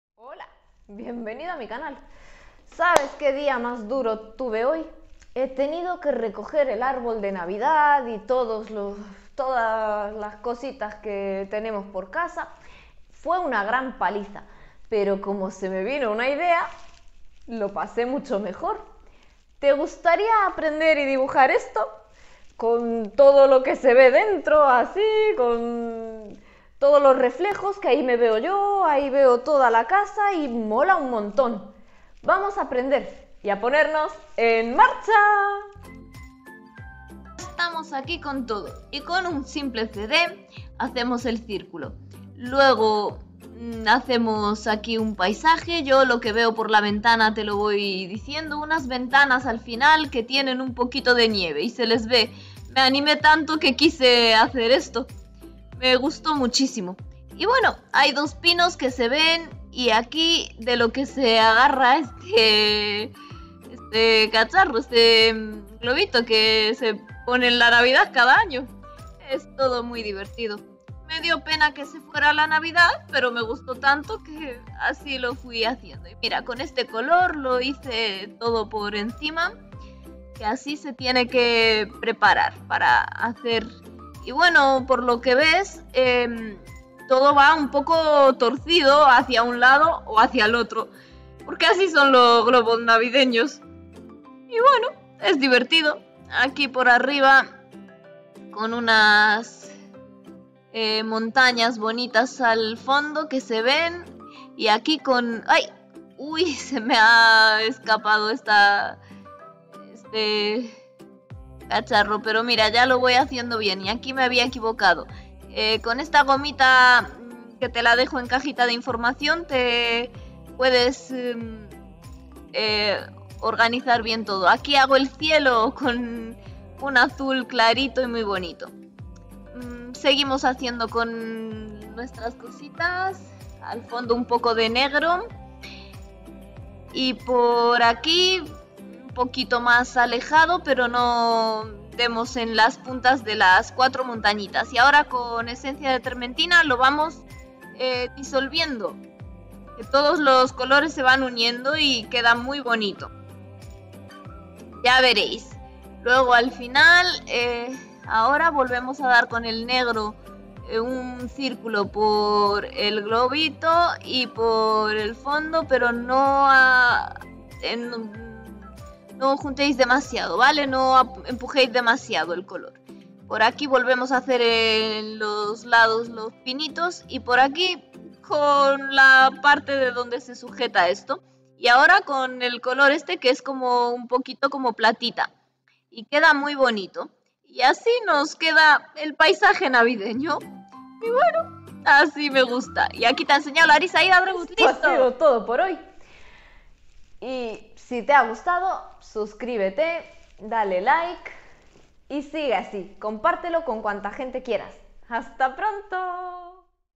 Cómo dibujar una bola de navidad con reflejos ambientales
Paro un dibujo así debes usar:
- lápiz 2b
- papel bristol
- un cd viejo
- lápices prismacolor premier: pc938, pc949, pc1087, pc1076, pc920, pc109, pc909 y pc1067